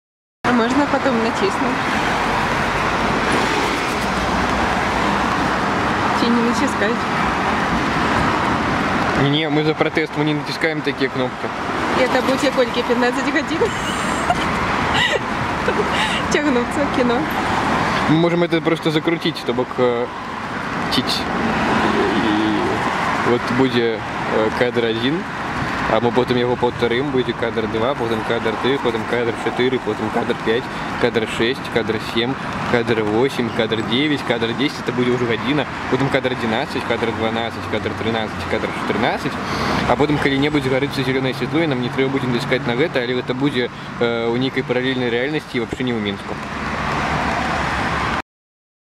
злы ўрбанізм
пераход дарогі - цэлая прыгода. як цудоўна, што існуе прылада, якая п̶е̶р̶а̶т̶в̶а̶р̶а̶е̶ ̶ж̶ы̶ц̶ц̶ё̶ ̶г̶а̶р̶а̶д̶ж̶а̶н̶ ̶у̶ ̶п̶е̶к̶л̶а̶  робіць штодзннае жыццё пешаходаў бяспечным, простым і нашмат цікавейшым. усяго адзін рух - націснуць кнопку, якой няма. ці задумваліся вы, як гэта працуе? вельмі ветлівая прылада са зваротнай сувяззю! яна дапаможа, калі вы спяшаецеся (не) альбо хочаце проста добра правесці час у прыемнай кампаніі, пакуль чакаеце зялёнага святла.

суаўтарка - Ірына Паўловіч